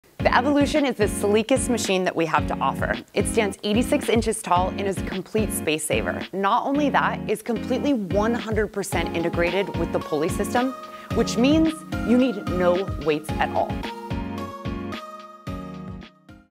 Jacked Up Power Rack Evolution Spotlight
Experience the Ultimate Workout with the Jacked Up Power Rack Evolution! 🌟

Welcome to our latest YouTube spotlight short, where we dive deep into the cutting-edge features of the Jacked Up Power Rack Evolution – the pinnacle of home gym engineering. This model stands apart with its fully integrated pin-select weight stack, seamlessly incorporated with both the pulleys and the smith machine. Say goodbye to the hassle of loading and unloading weight plates; with the Power Rack Evolution, achieving an intensive, full-body workout has never been easier or more convenient.

🔥 Key Highlights:

Integrated Weight Stack: Enjoy the ease of pin-select adjustments on both the pulleys and the smith machine, designed for a smooth, efficient workout experience.
No Plates Required: Eliminate the need for a separate weight collection. The built-in weight stack caters to all your lifting needs.
Versatile Workouts: Whether you're targeting strength, endurance, or muscle building, the Power Rack Evolution is equipped to support a wide range of exercises.
Space Efficiency: With everything you need in one compact, robust unit, make the most of your home gym space without compromising on functionality.
The Power Rack Evolution is not just a piece of equipment; it's your all-in-one solution to a perfect home workout. Engineered for both beginners and seasoned athletes, it promises to deliver an unparalleled exercise experience, emphasizing safety, convenience, and versatility.

🔗 Check out the Jacked Up Power Rack Evolution and elevate your home workouts to legendary status. Your journey to peak fitness starts here!